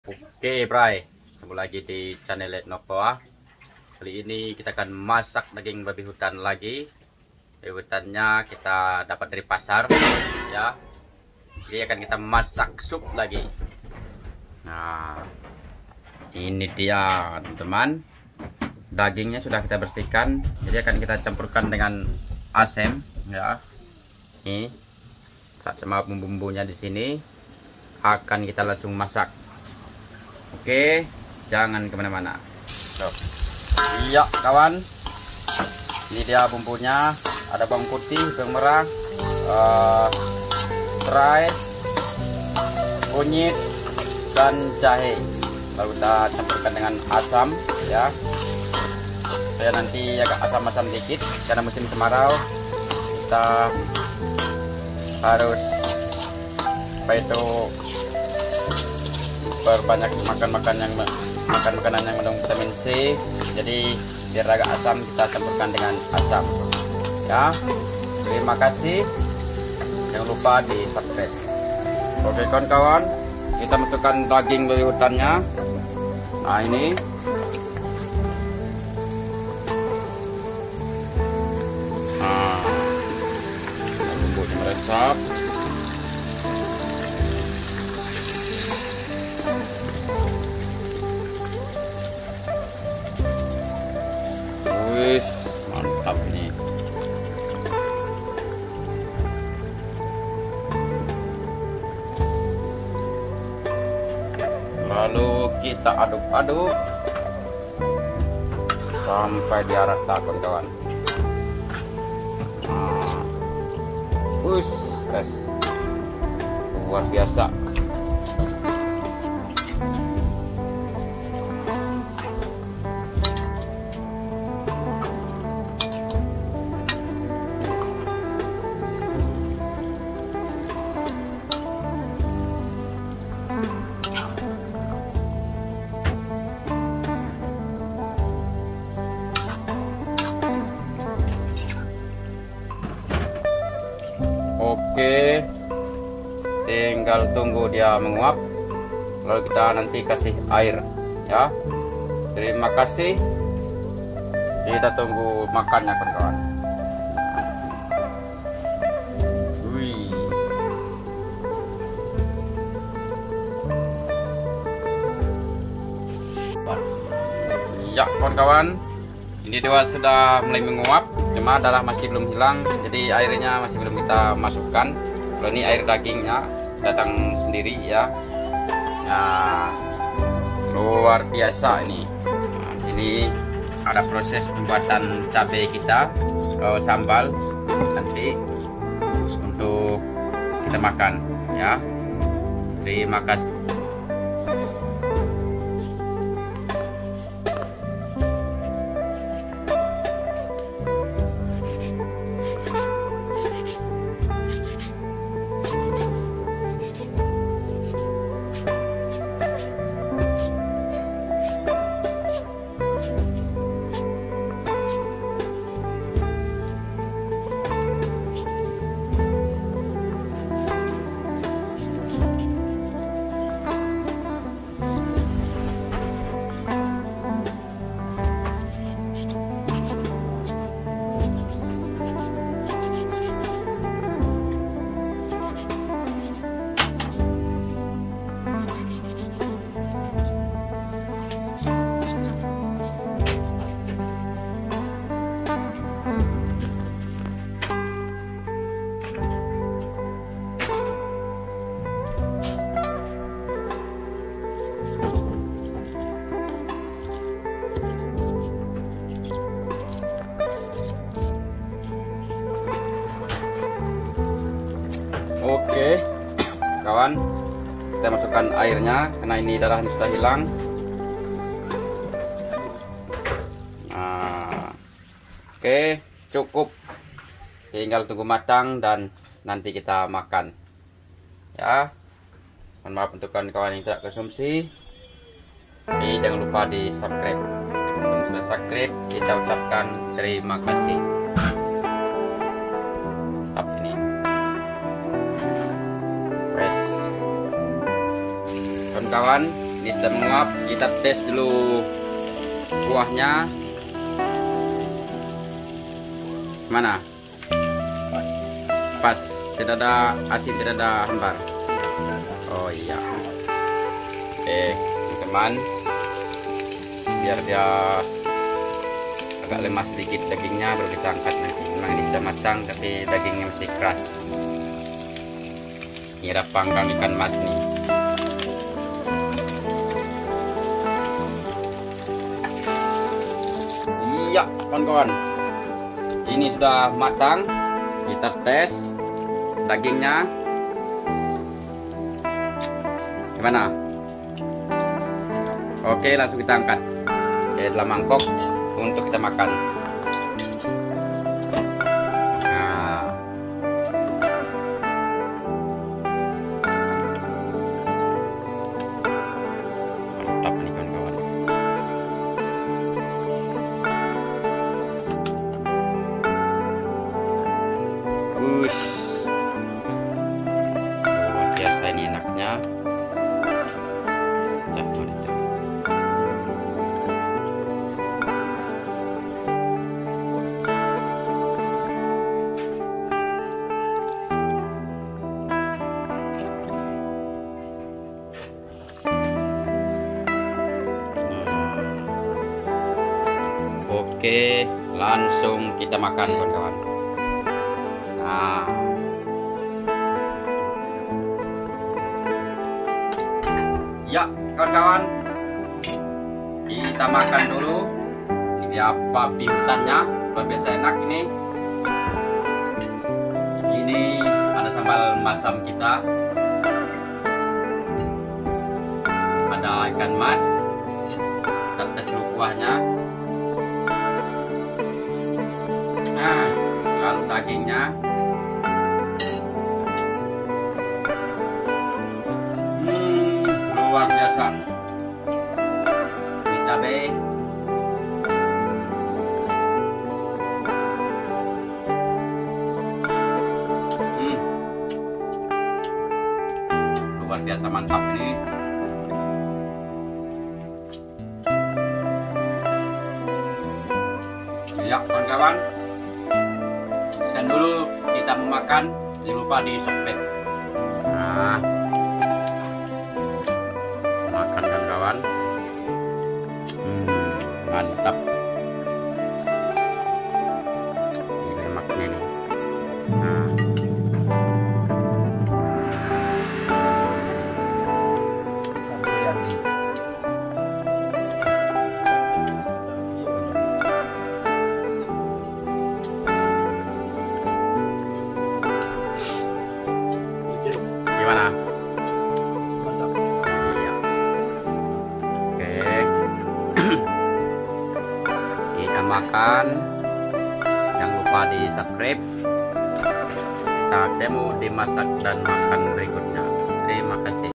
MASAK DAGING BABI HUTAN DI DAPAT DARI PASAR
selamat pagi kawan,,,kita masak babi hutan lagi ya,,,,,